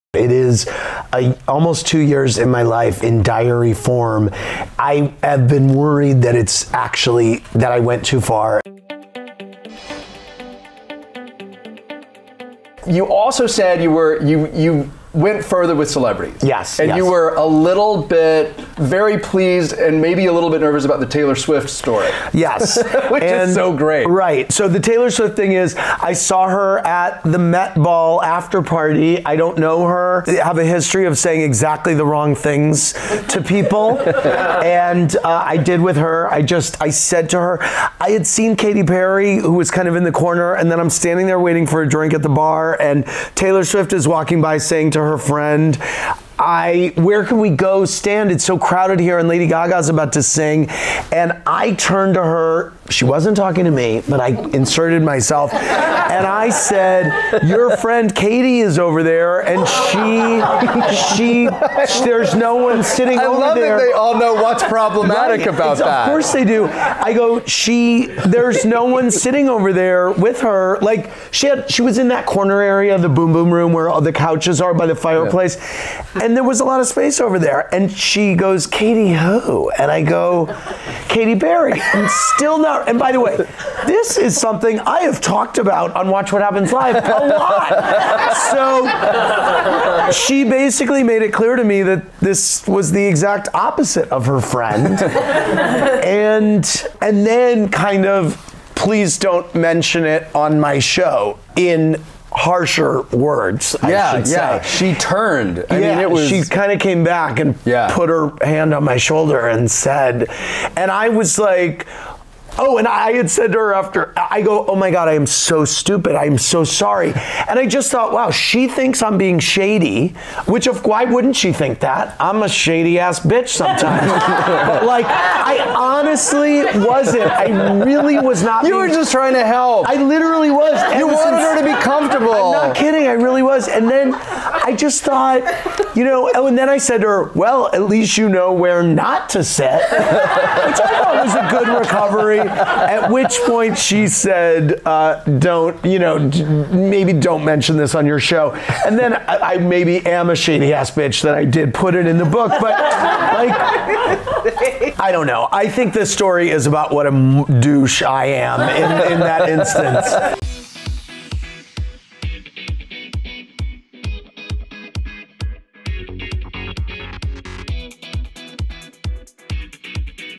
Andy Cohen's Cringeworthy Encounter with Taylor Swift | PEN | Entertainment Weekly
Watch What Happens Live' host Andy Cohen sits down with Jess Cagle to talk about his book 'Superficial', Taylor Swift & much more.